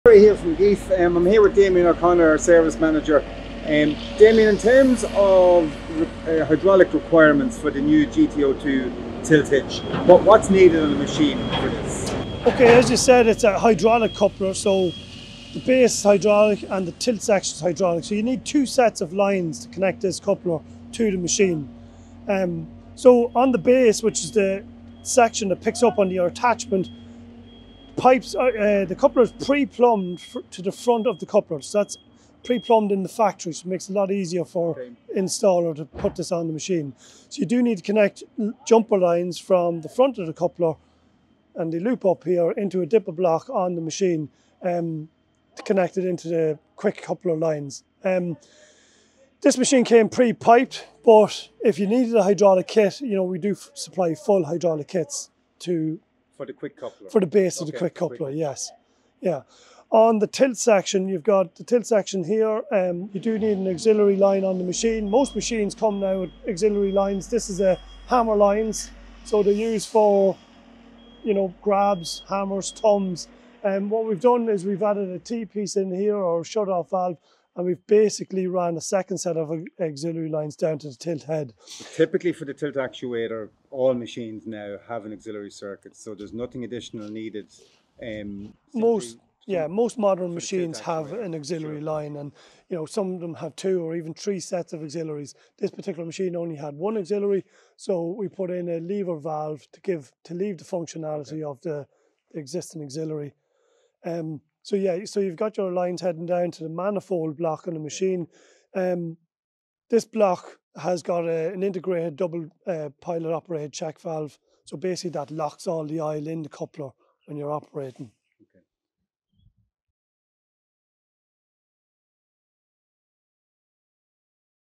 What do you need to Install & Operate a Geith Tilting Quick Coupler?
This is a frequently asked question about the installation and operation of the Geith Tilting Quick Coupler. Here’s what Damien O'Connor has to say: